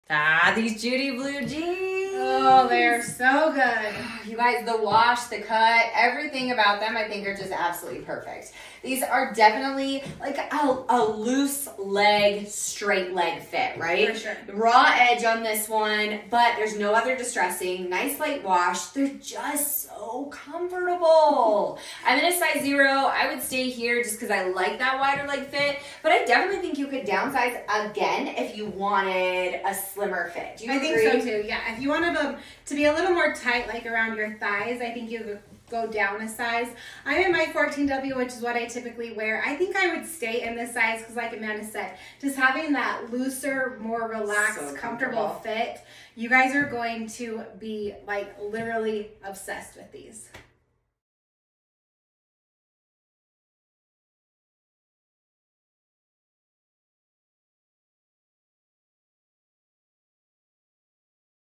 Judy Blue Jeans from Hazel Blues®
We are one of the largest Judy Blue retailers in Utah.  Come find your perfect fit!

Look great and feel your best in our Tate High Rise Straight Jeans! Crafted in a 4-way stretch for comfort and style, these jeans feature a rib cage high rise and throwback straight cut, with a light wash and destroyed hem for a vintage-inspired look. Elevate your wardrobe with these unique jeans!

Judy Blue
Rib Cage High Rise
Zip Fly
4-Way Stretch
Straight Silhouette
Distressed Raw Hem
66% Cotton, 21% Polyester, 11% Rayon, 2% Spandex
True to Size
Model info:
Amanda in size 0. Hips 38", Waist 26", Height 5'6"
Kirra in size 14W. Hips 50", Waist 39" Height 5'6".